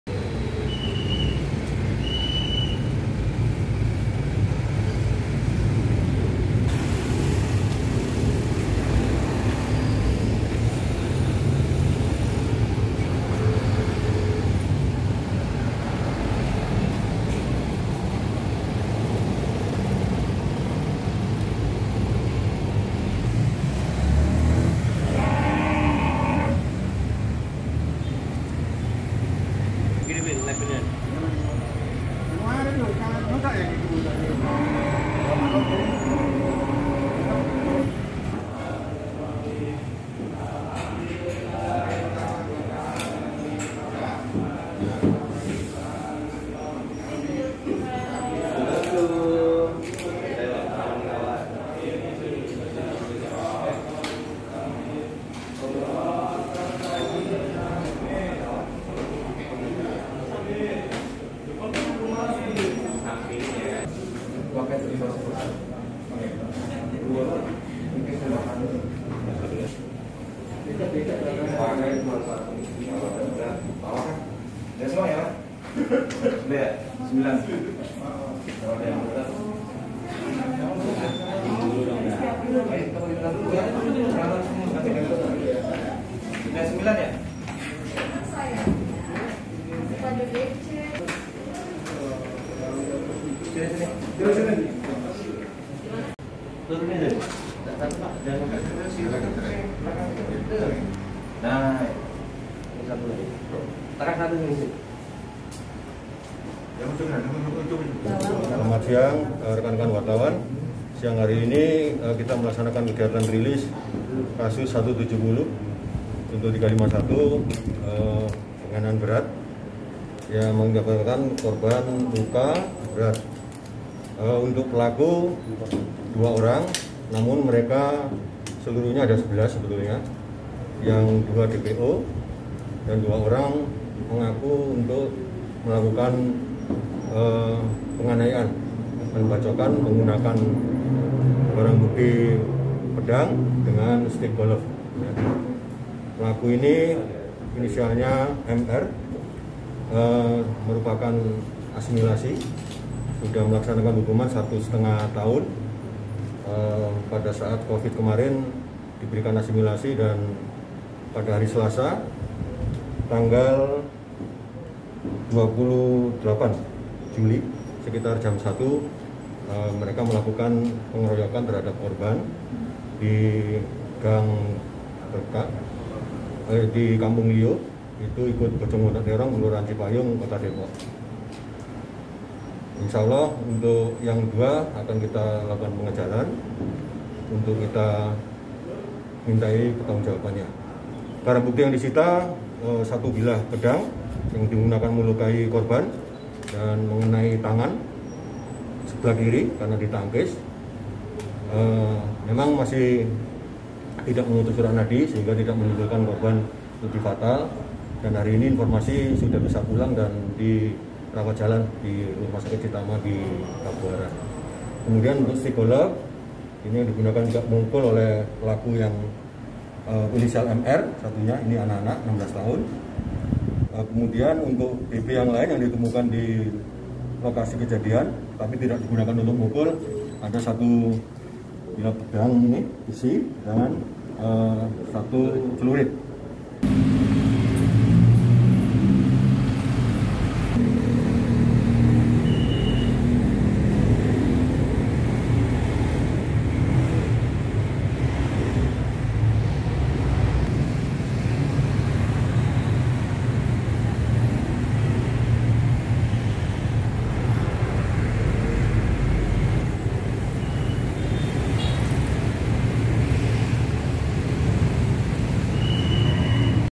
Polisi Amankan Pelaku Tawuran Yang Bacok Tangan Korban di Depok
Polsek Pancoran Mas berhasil mengamankan sembilan pelaku tawuran yang menimbulkan korban luka parah di bagian tangan kirinya.

Luka tersebut diakibatkan tebasan senjata tajam jenis pedang yang membuat urat nadi korban putus.

Kepala Polsek Pancoran Mas (Kapolsek) Kompol Tri Harijadi mengatakan, ke sembilan pelaku tersebut ada yang masih berusia di bawah umur.

"Seluruhnya ada 11 orang tapi yang dua masih DPO dan dua lagi mengaku melakukan penganiayaan dengan barang bukti pedang dan stik golf," tutur Tri kepada wartawan di Polsek Pancoran Mas, Kota Depok, Kamis (30/7/2020).

Dari dua orang pelaku pembacokan, seorang diantaranya adalah napi yang pada saat Covid-19 mendapat asimilasi atau pembebasan besyarat untuk mengurangi kepadatan di dalam ruang tahanan.

"Pelaku ini inisialnya MR (19) merupakan pelaku asimilasi. Pada Selasa (28/7/2020) sekitar jam 1 (dini hari) di Gang Berkah, Kampung Lio, melakukan pembacokan. Insya Allah untuk yang dua (DPO) kita lakukan pengejaran," papar Tri.

Tri mengatakan, kronologi awalnya korban dan pelaku bersama teman-temannya saling janjian melalui aplikasi whatsapp untuk tawuran.

Namun sebelum menyepakati letak lokasi tawuran, pelaku sudab lebih dulu menyambangi korban dan melakukan pembacokan.

"Janjian awalnya untuk tawuran namun karena pelaku sudah tahu posisi korban dimana, kemudian disamperin dan didatangi lalu kemudian dilakukan pembacokan," kata Tri.

Dari hasil penangkapan terhadap para pelaku, Polsek Pancoran Mas berhasil mengamankan sejumlah barang bukti yakni satu buah celurit, satu buah pedang, satu buab besi tipis yang bagian ujungnya tajam, sebuah stik golf, serta dua buah sepeda motor yang digunakan untuk menyambangi korban.

Satu buah pedang yang digunakan pelaku untuk membacok pun bertuliskan kaligrafi yang dikatakan Tri bertujuan agar tindakan pelaku dapat berjalan lancar.

"Tulisan ini katanya biar saat menggunakannya bisa sukses melukai korban, dari pengakuan pelaku baru satu kali menggunakan pedang ini dan berhasil (melukai korban)," ujar Tri.

Saat ditanya, MR mengaku tidak mengetahui arti dari tulisan kaligrafi yang ada di pedang tersebut.

"(Pedangnya) punya teman, saya ambil itu di pasir karena disimpannya di pasir. Baru satu kali saya pakai, ngga tahu (arti dan bacaannya) apa," papar MR.

Reporter : Wartakotalive.com / Vini Rizki Amelia
Editor Video : M.Rusdi